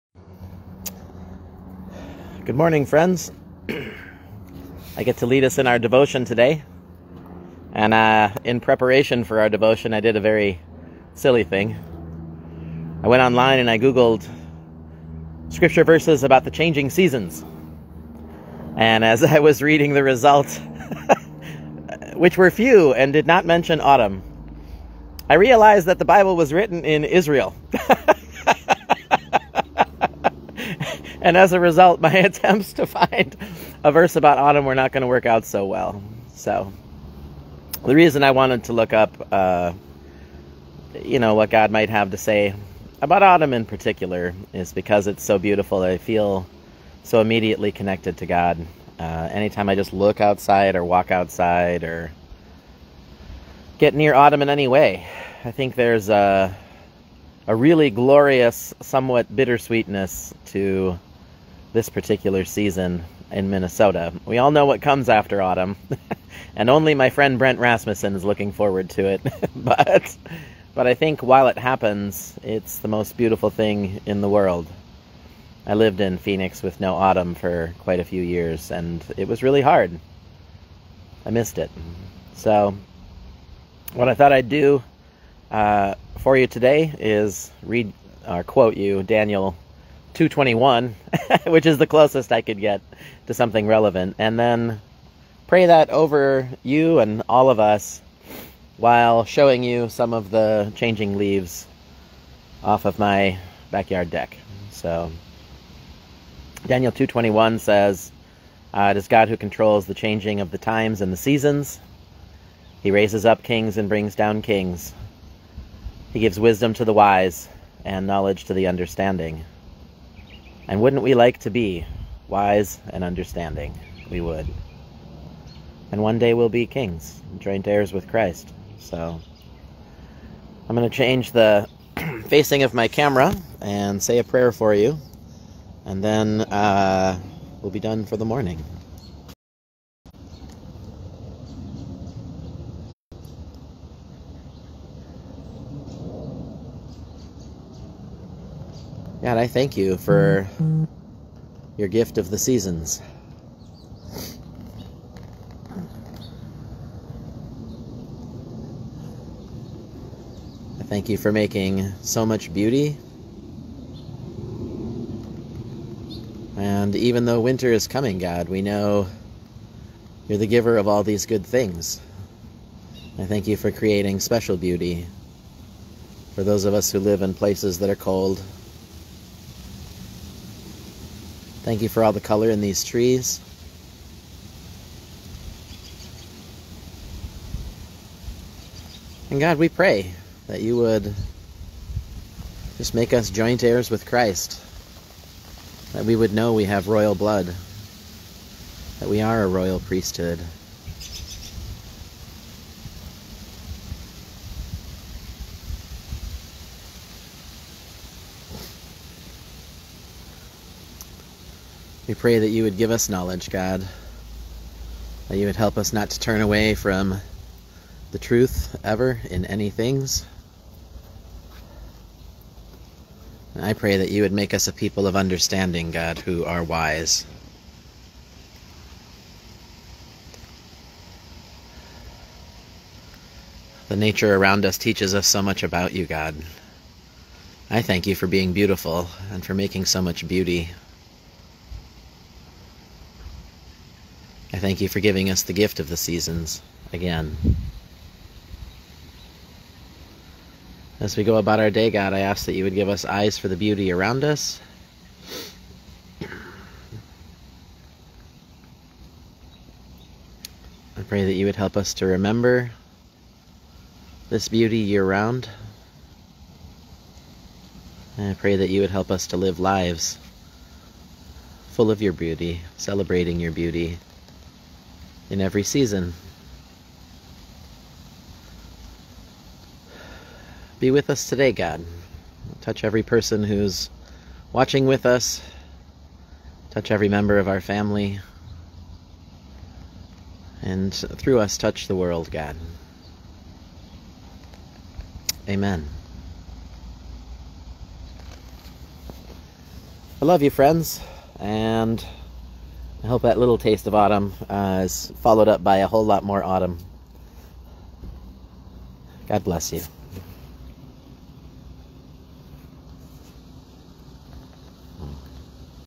Monday Devotion - October 10, 2022 - Peter Benedict
Join Peter for our Monday devotion, and enjoy the Autumn beauty.